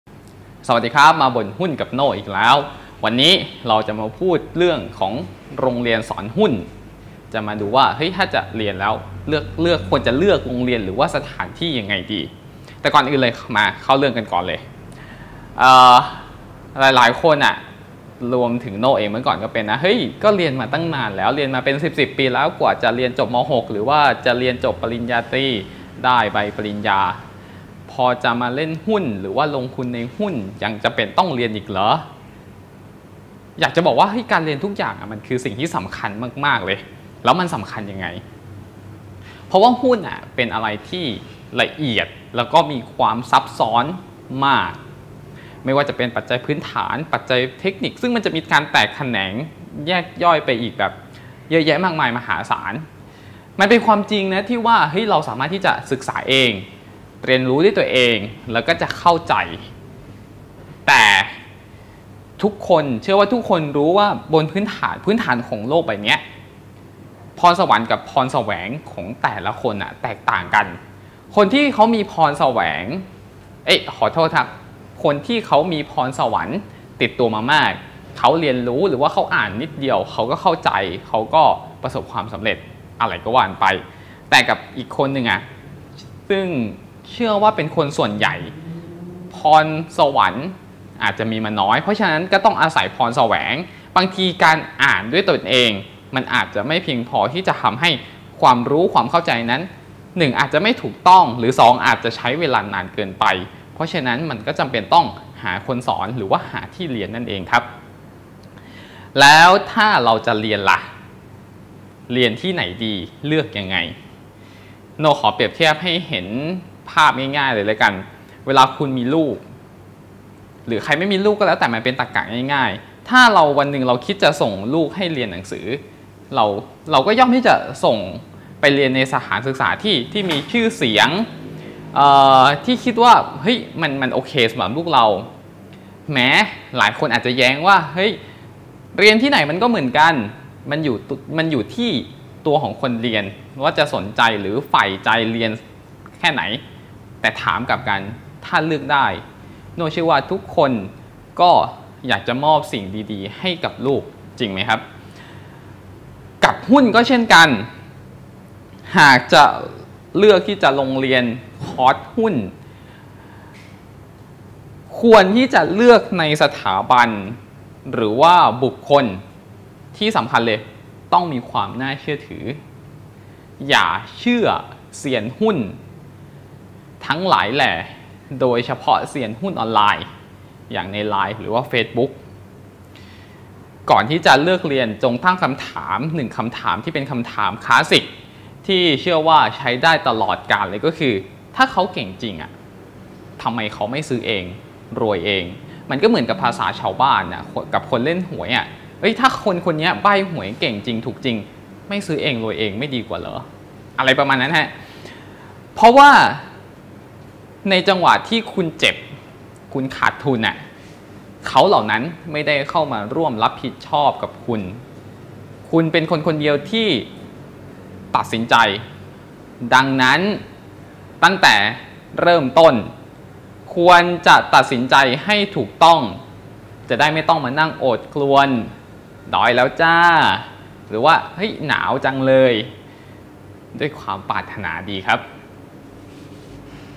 บ่นหุ้นกับโน่ 11 "โรงเรียนสอนหุ้น"
เรียนตั้งแต่เด็ก เรียนเป็นสิบ ๆ ปี กว่าจะได้ใบปริญญา จะลงทุนในหุ้นยังต้องเรียนอีกหรือ?

โดย แว่นใส